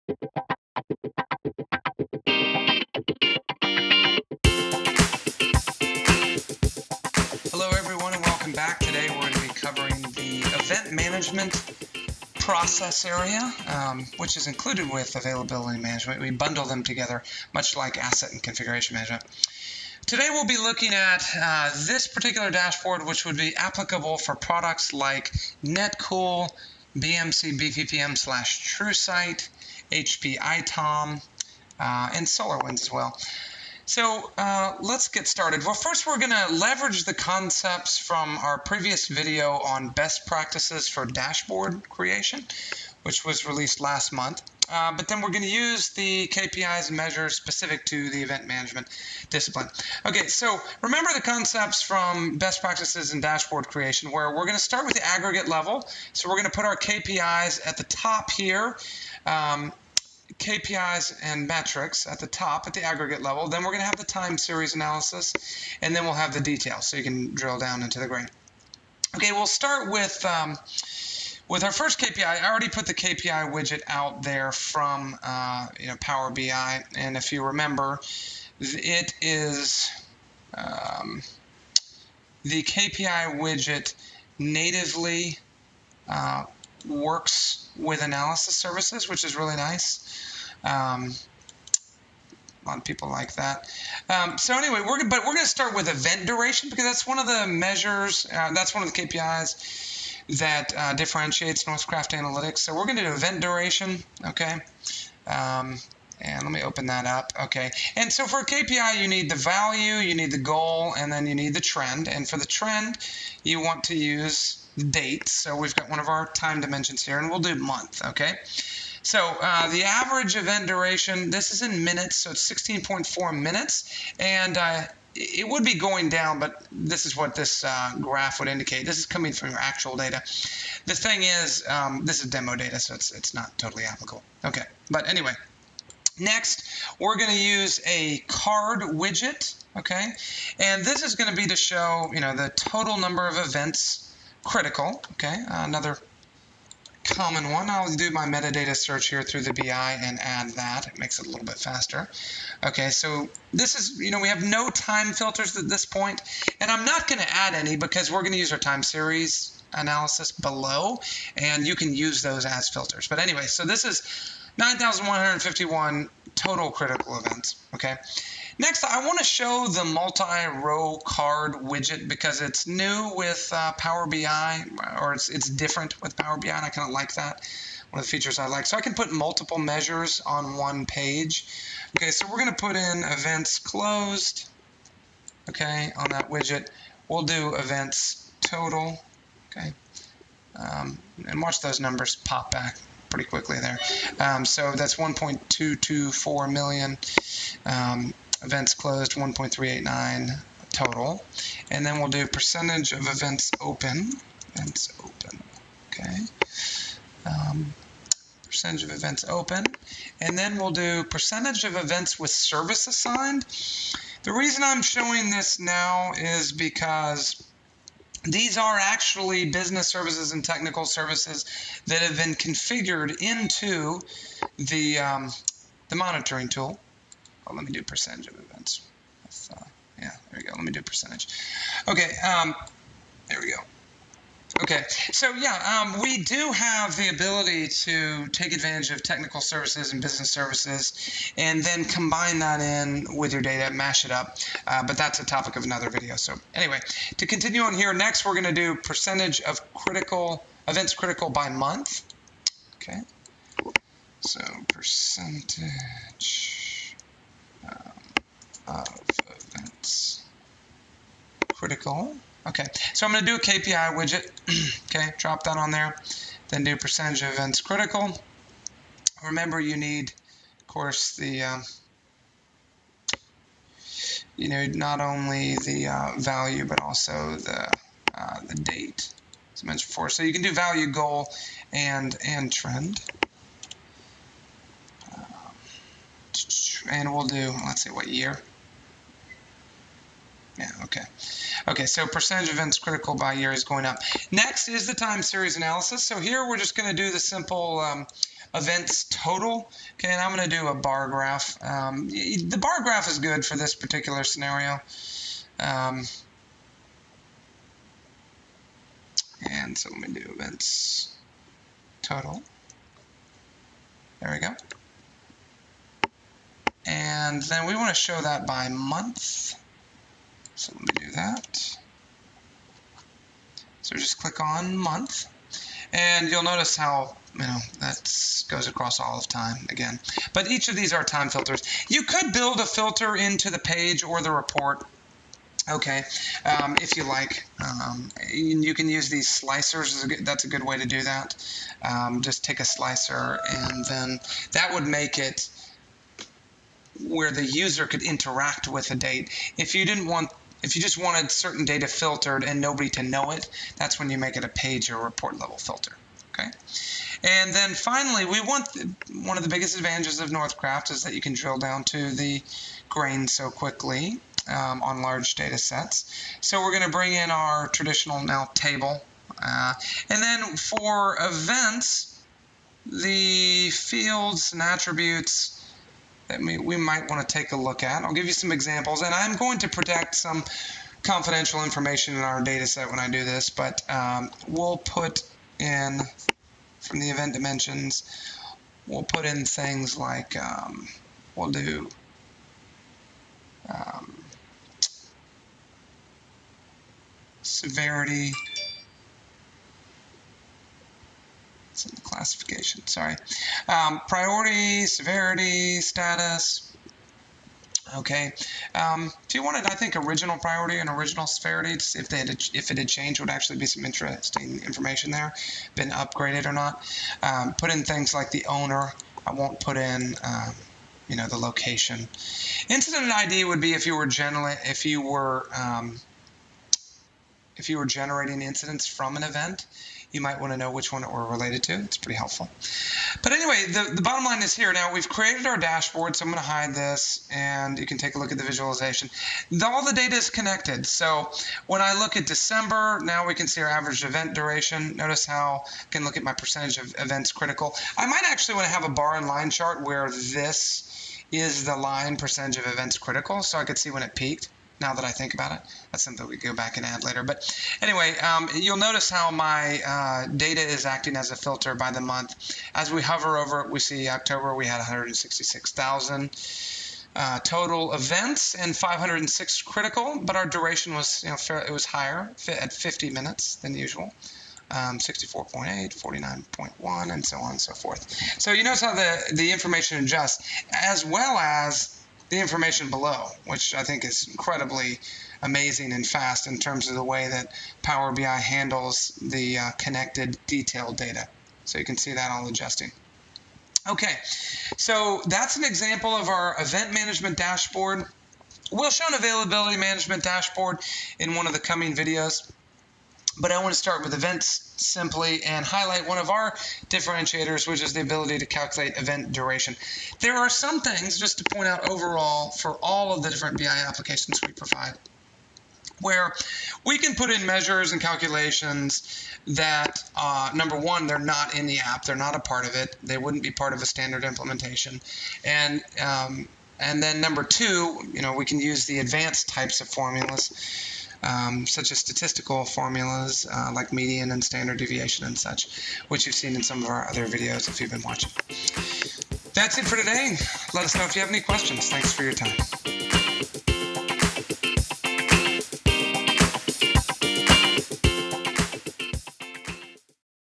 Event & Availability Analytics
In this video, we'd like to show you how to harness the power of a larger data set to show you how to find the critical infrastructure "needles" in your ITOM "haystack" of events.